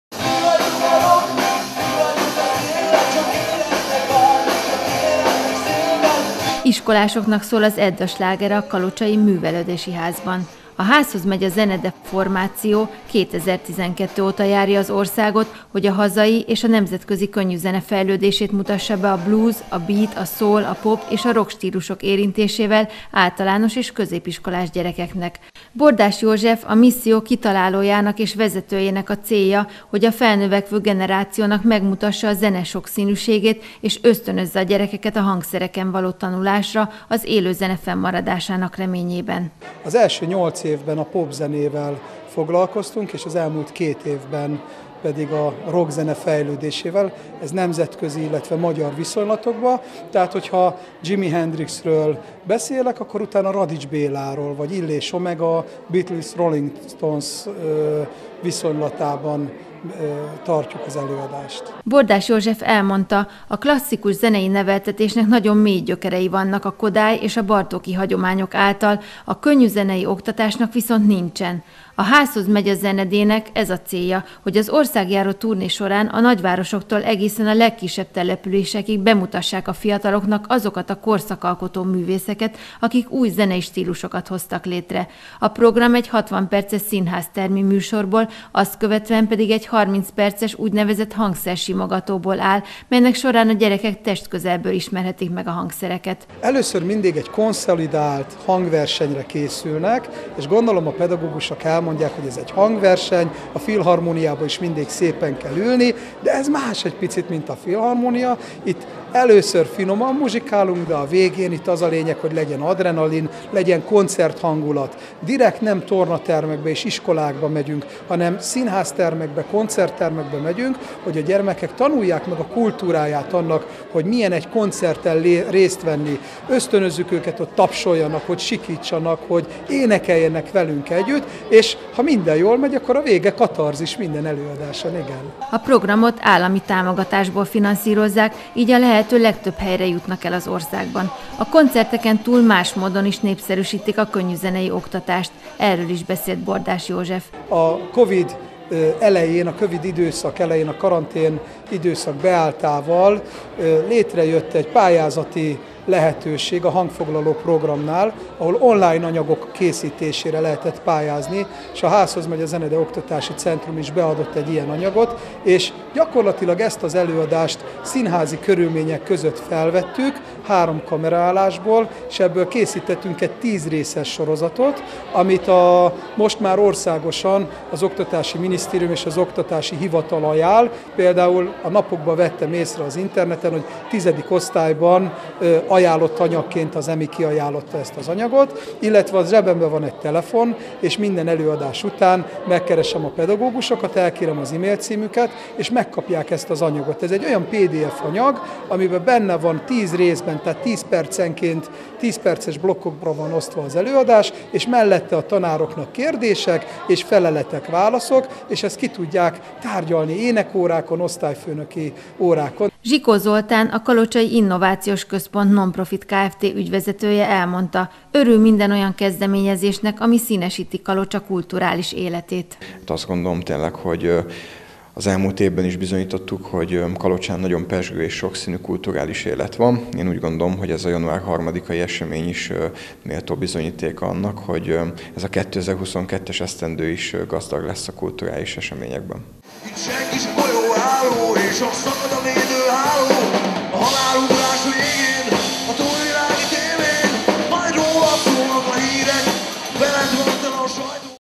Zeneoktatás koncerthangulatban
A Házhoz megy a Zenede formáció tematikus sorozat formájában mutatja be a rock zene hazai és a nemzetközi megteremtőit. Jimi Hendrix, Radics Béla, Illés, Omega, Rolling Stones és még számos ikonikus zenekar és rock zenész dalait adják elő színháztermi körülmények között. Az előadások általános és középiskolások részére szólnak, a cél az, hogy ablakot nyissanak a gyerekek számára a zene sokszínűségére és ösztönözzék őket a hangszereken való tanulásra.